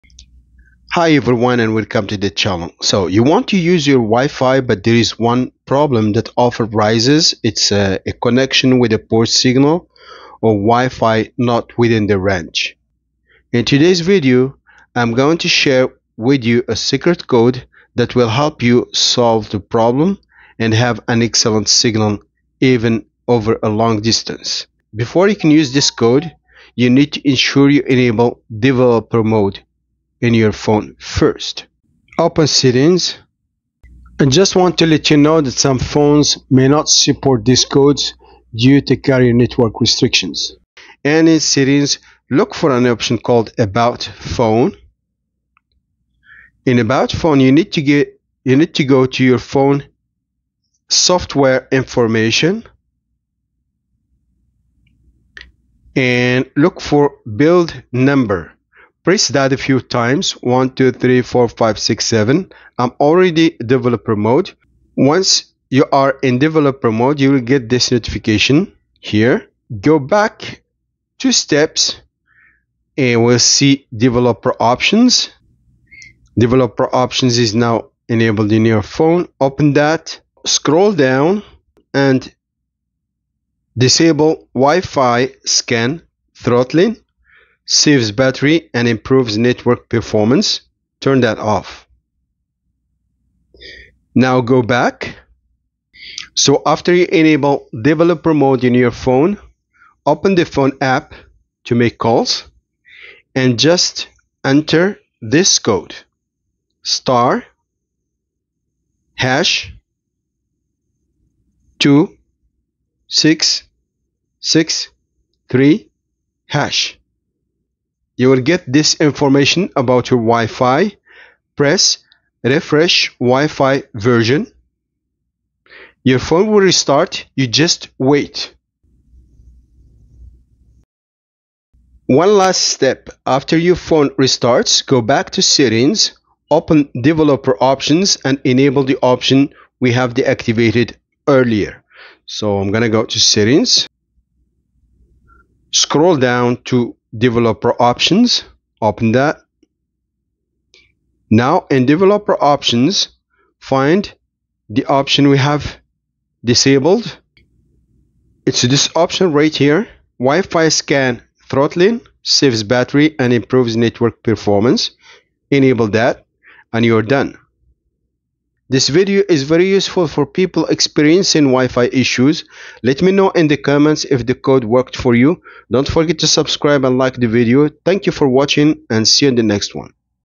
Secret WIFI Code You Absolutely Need to Know in 2025
In today's video, I am going to show you how to use a secret wifi code in your Android phone to fix common Wifi issues and improve wifi signal: Poor reception, weak or fair signal.